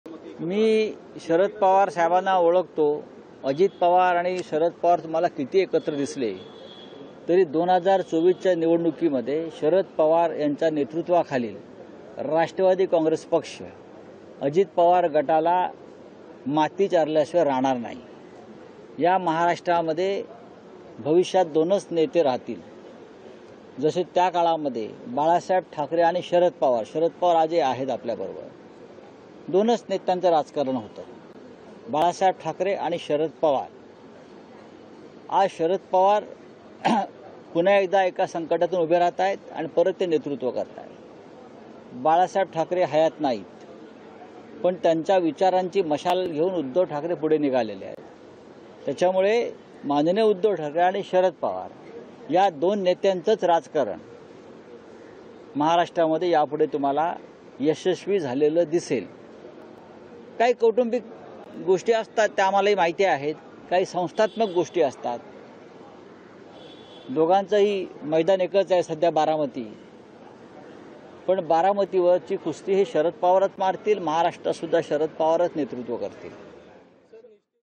Sanjay Raut On Sharad Pawar | बारामतीत शरद पवार अजित पवारांना धूळ चारतील | DvM
बारामतीत शरद पवार अजित पवारांना धूळ चारतील: संजय राऊतांचा दावा, म्हणाले- महाराष्ट्रात निवडणुकीनंतर दोनच नेते उरतील